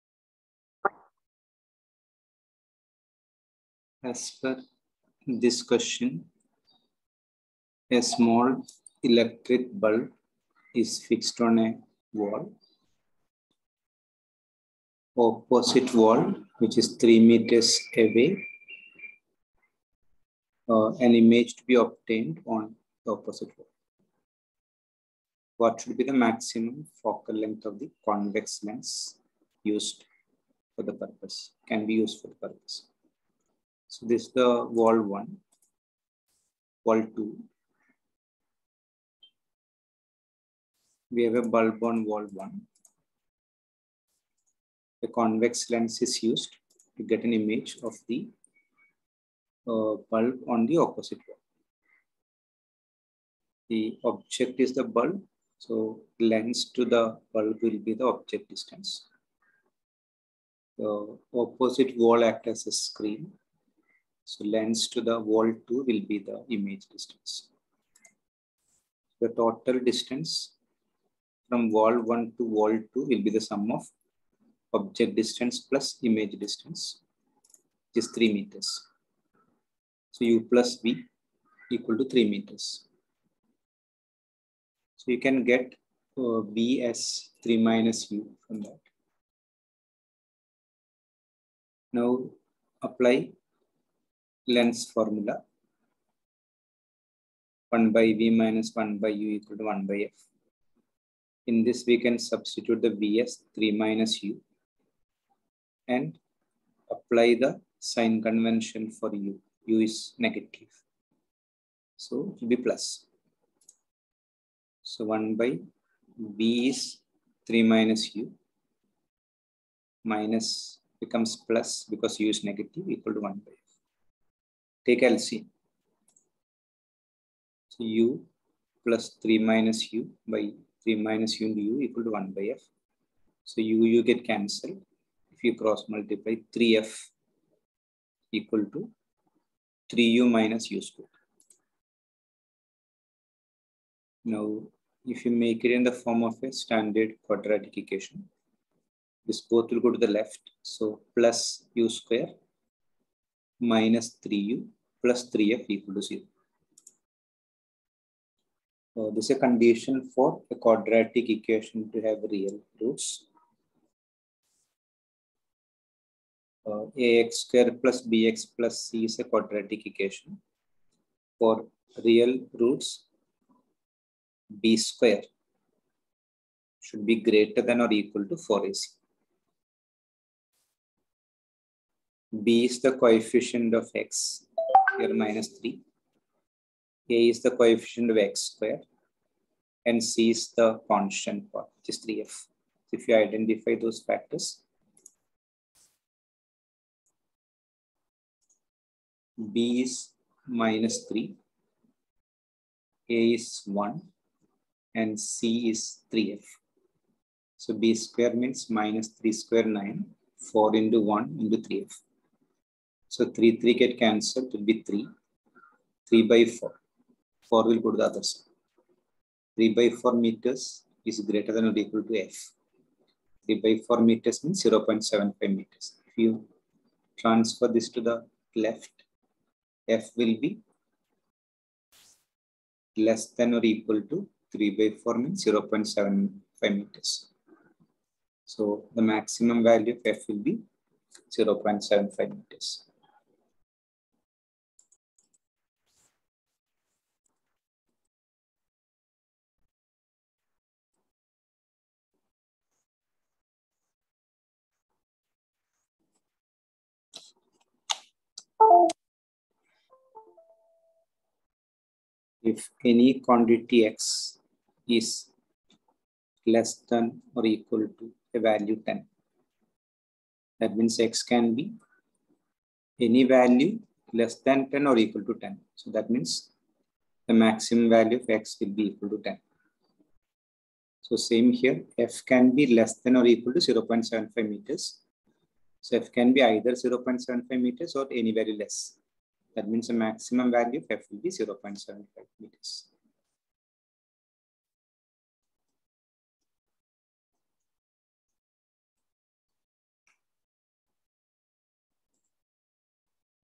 GRADE XII TERM II RAY OPTICS MAXIMUM FOCAL LENGTH
The image of a small electric bulb fixed on the wall of a room is to be obtained on the opposite wall 3m away from a means of a large convex lens. What is the maximum possible focal length of the lens required for the purpose?